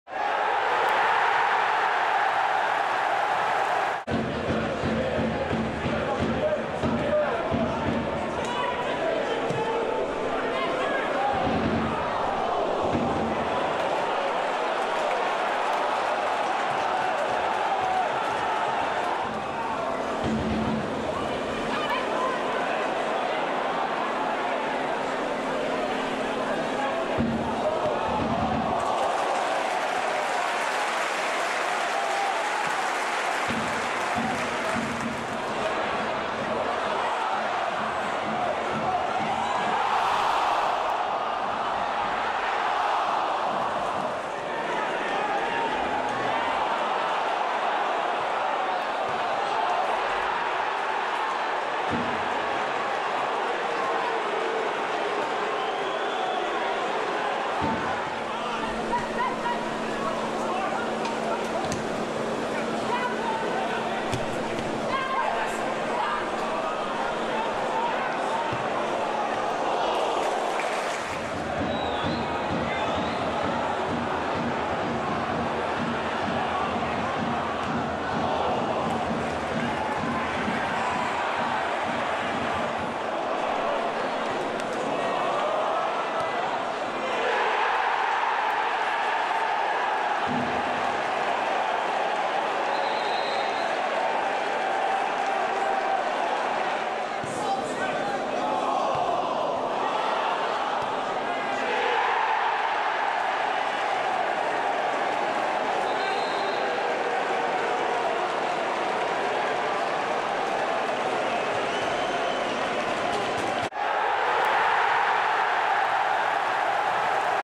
HIGHLIGHTS: CHARLTON ATHLETIC 0 WIGAN ATHLETIC 3 - 12/09/2017
Match highlights from the Valley where Latics beat Charlton Athletic 3-0 on Tuesday night thanks to two goals from Gavin Massey and a strike from Sam Morsy.